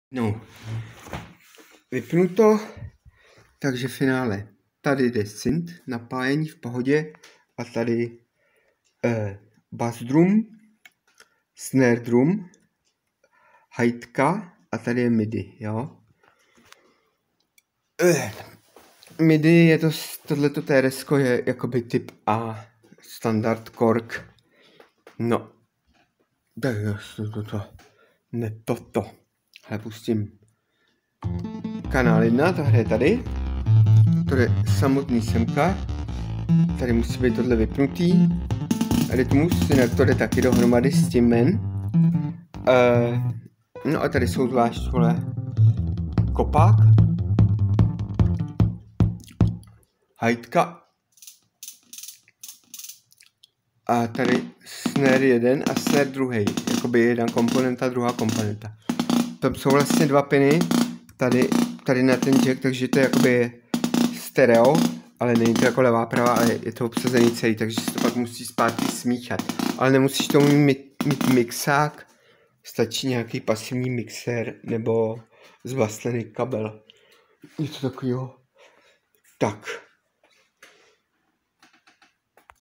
Monotribe mod MIDI in + individual outs done
Custom modification is done, this is a test and explain.